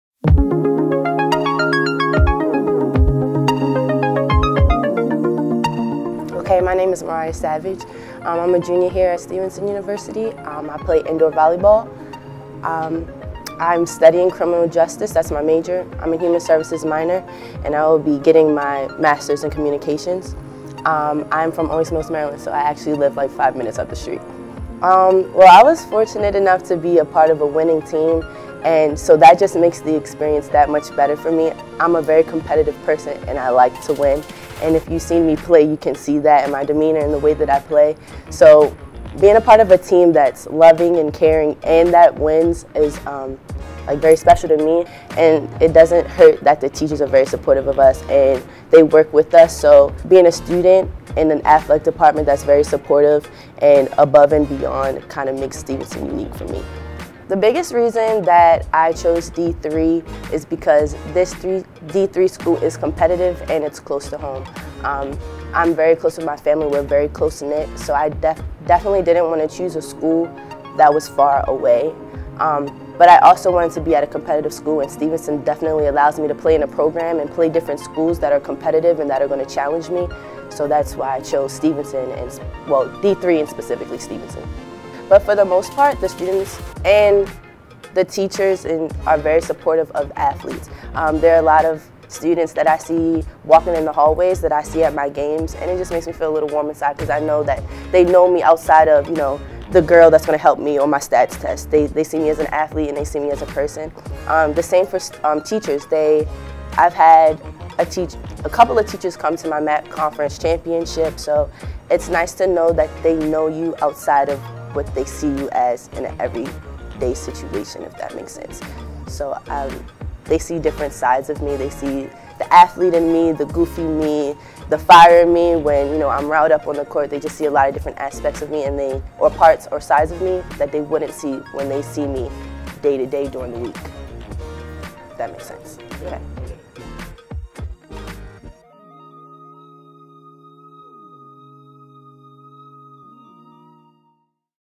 Inside the Stable - Episode 6
Women's volleyball junior Moriah Savage talks about the unique qualities Stevenson possesses and the opportunities Division III has allowed her to achieve.